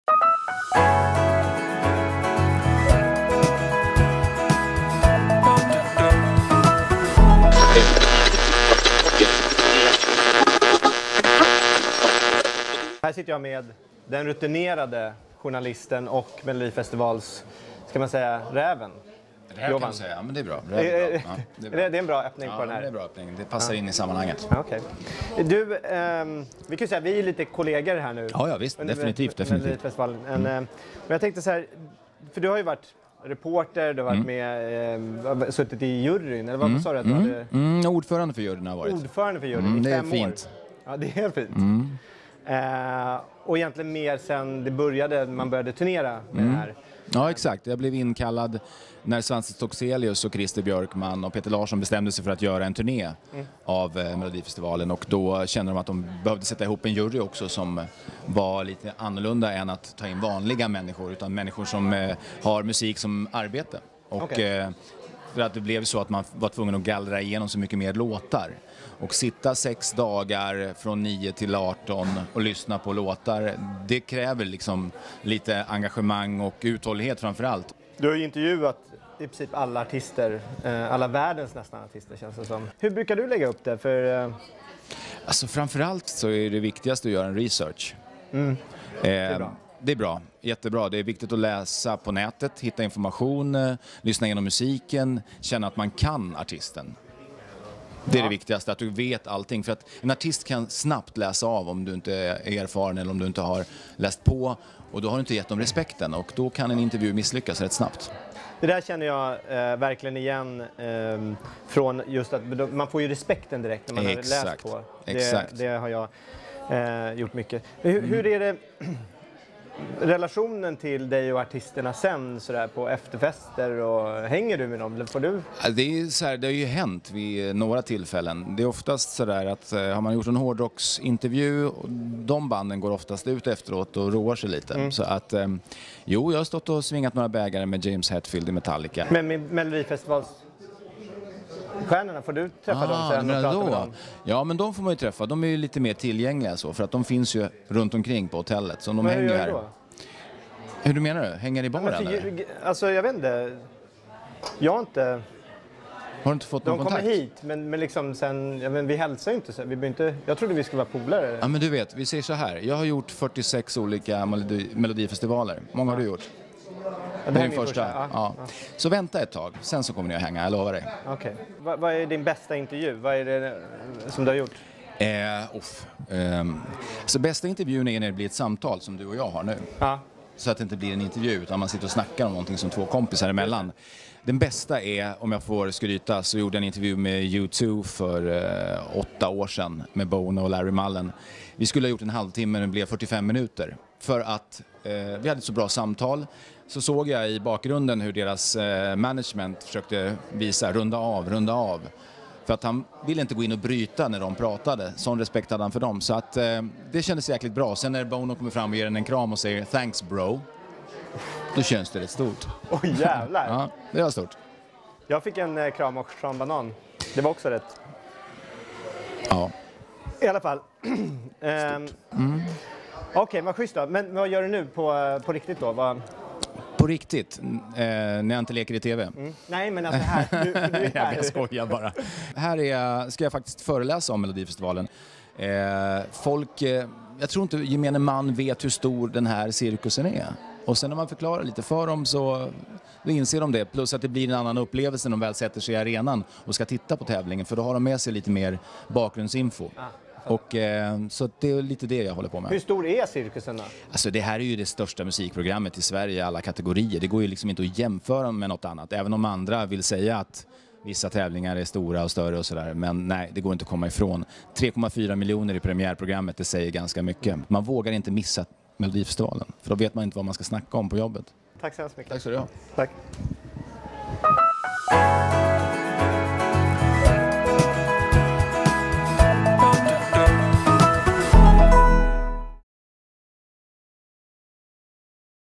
Jovan Radomir på Melodifestivalen 2012 berättar hemlisar!
Journalisten Jovan Radomir har varit inblandad i 46 Melodifestivaler och Eurovision Song Contest, varit ordförande för juryn och dessutom och träffat många internationella stjärnor. T ex James Hetfield - Metallica, Bono och Larry Mullen - U2.
Här lär han ut hur man kan bli kompis med stjärnorna och berättar om hur stort Melodifestivalen är.